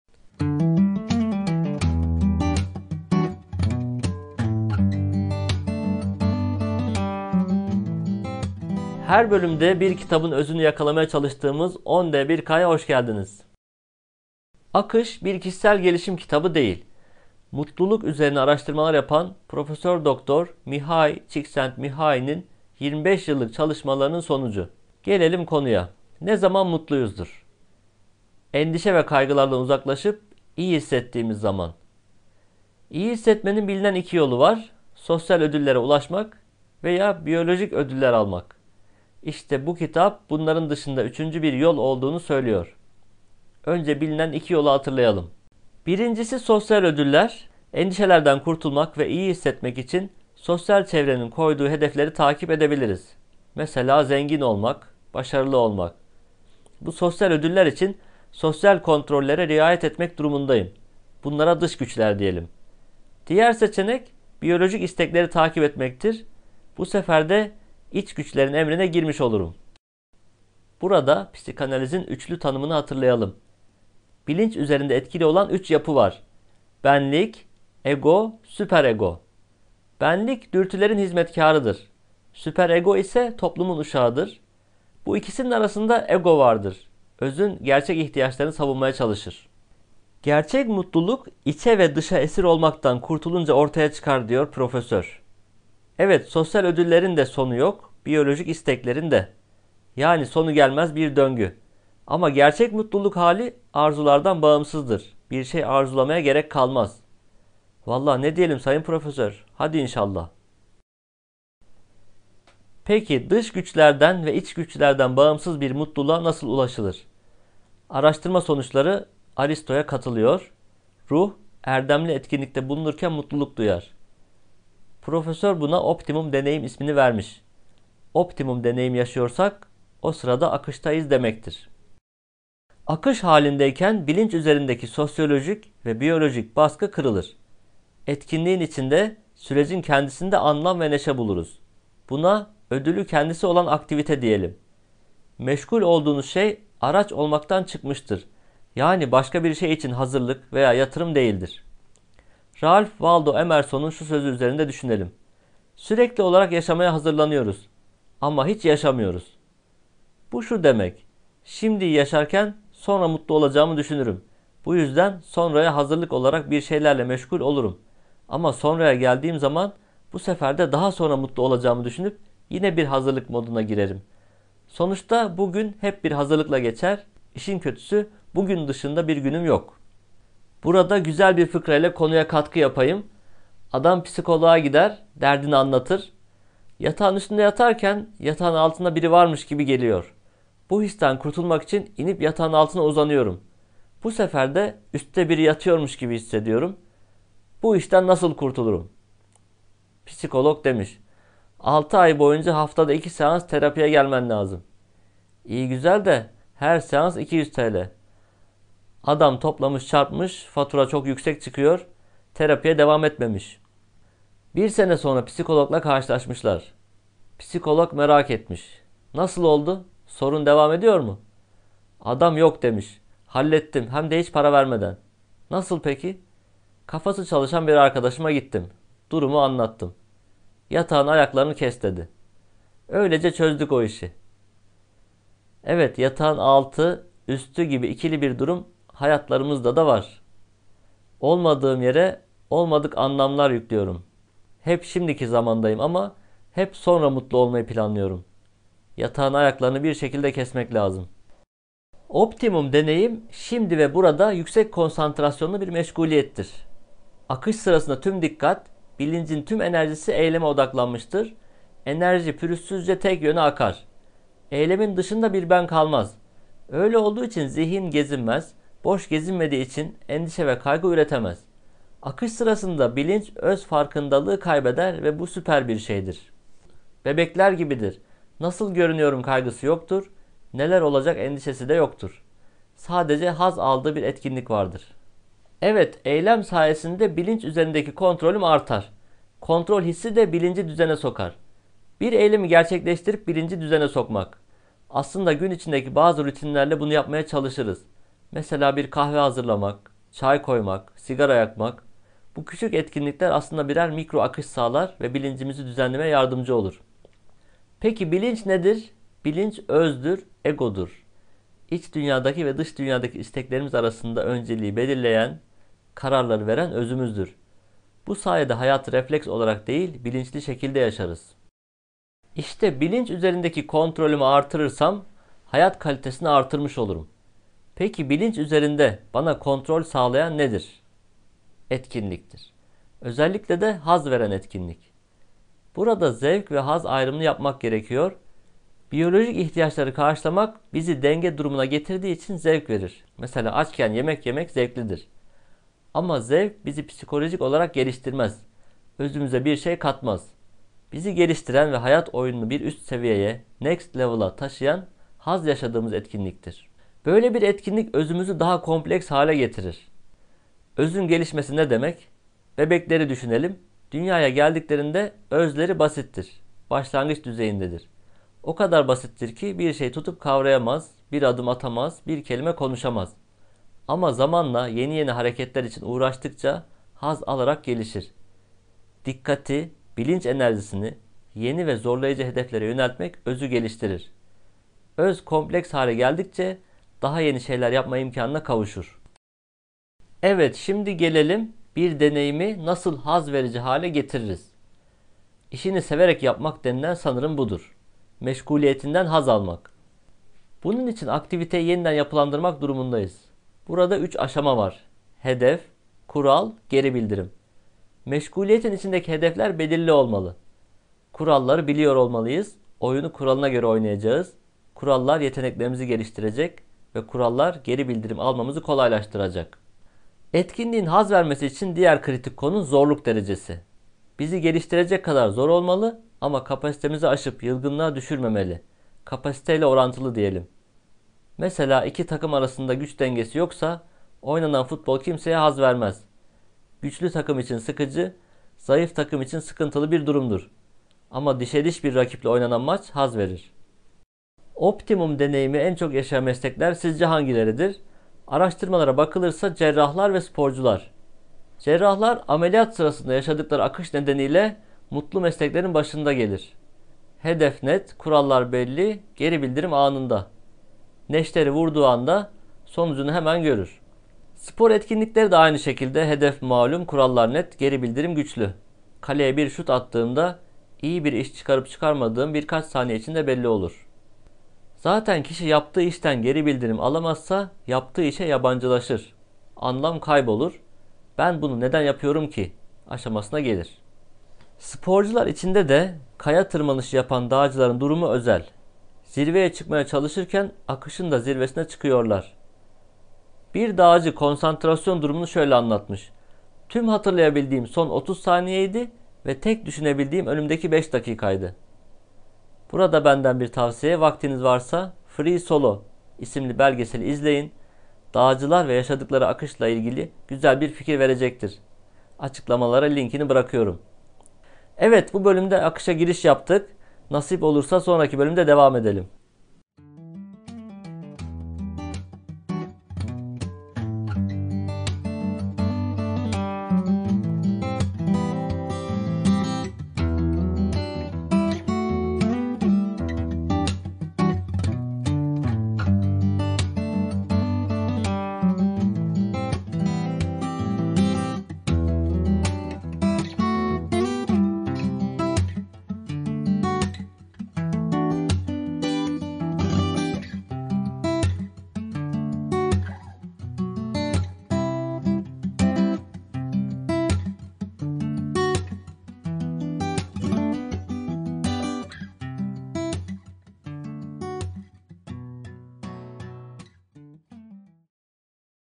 Akış - Mutluluk Bilimi - Sesli Kitap Özeti
Akış, bir kişisel gelişim kitabı değil. Mutluluk üzerine araştırmalar yapan Prof. Dr. Mihaly Csikszentmihalyi 'nin 25 yıllık çalışmalarının sonucu. Gelelim konuya. Ne zaman mutluyuzdur ? Endişe ve kaygılardan uzaklaşıp iyi hissettiğimiz zaman. İyi hissetmenin bilinen iki yolu var, sosyal ödüllere ulaşmak veya biyolojik ödüller almak. İşte bu kitap, bunların dışında üçüncü bir yol olduğunu söylüyor.